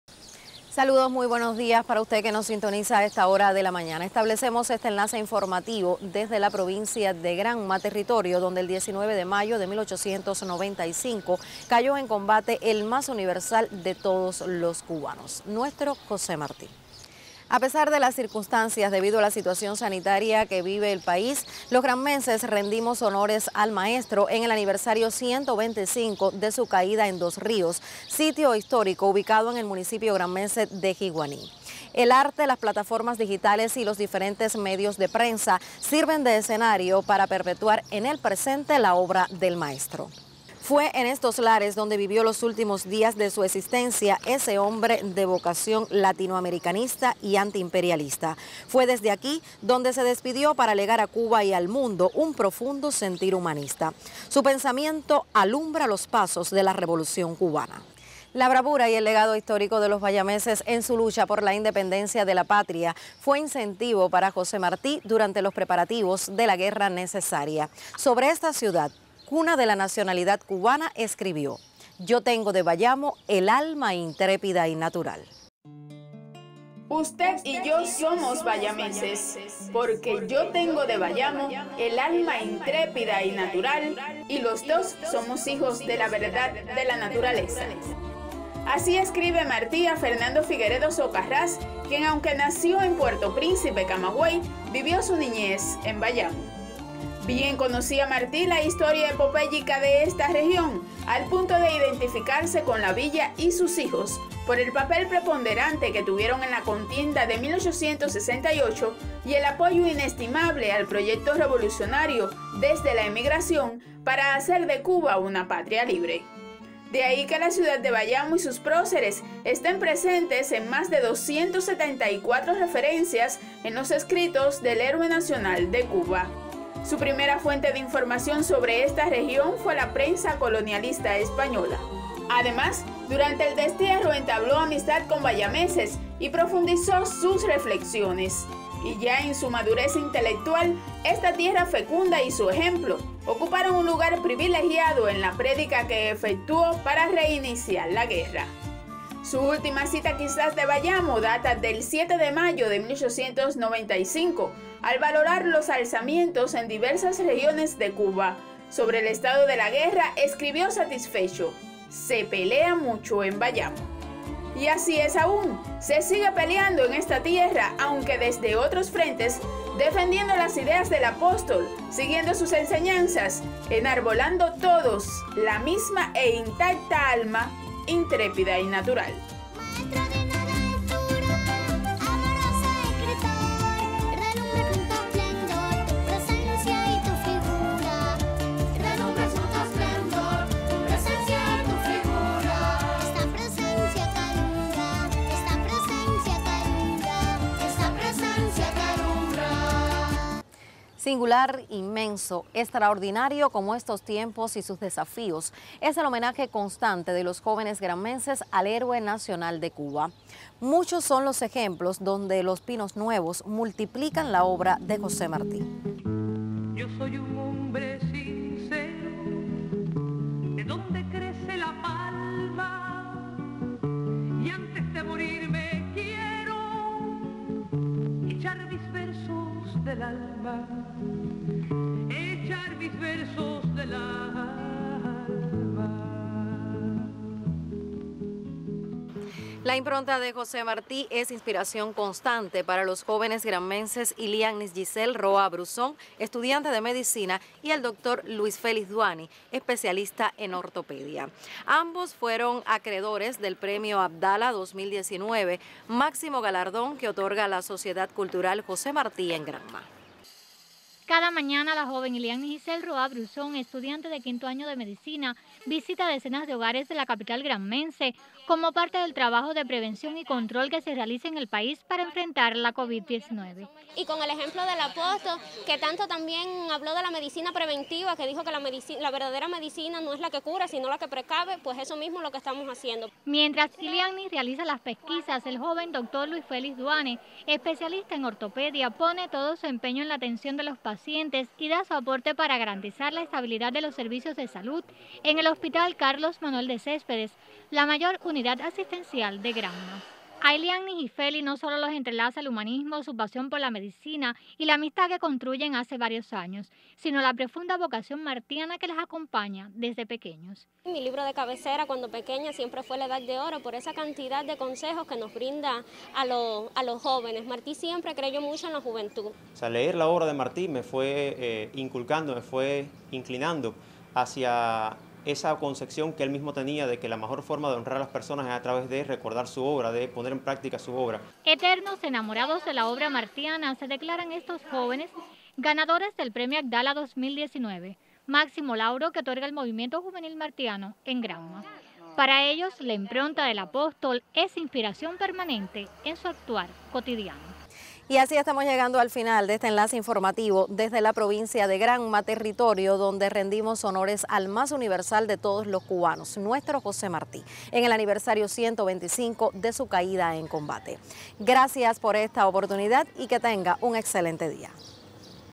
Conmemoran en Granma el aniversario 125 de su caída en combate de José Martí
A pesar de las circunstancias, debido a la contingencia sanitaria que enfrenta el país, los granmenses rendimos honores al apóstol en el aniversario 125 de su caída en Dos Ríos, sitio histórico ubicado en el municipio de Jiguaní.  El arte, las plataformas digitales y los medios de prensa sirven de escenario para perpetuar desde el presente al más universal de los cubanos.